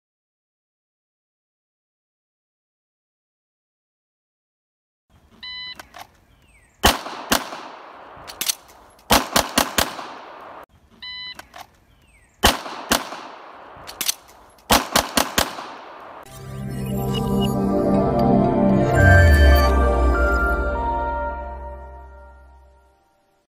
Cold FAST In Richmond Class
One of the students in my Richmond, VA class asked me to demo a FAST at the lunch break. This was done cold and I was unaware he was filming. I show it twice to show the times recorded by ClipShot (which won't let me flip the video in the app).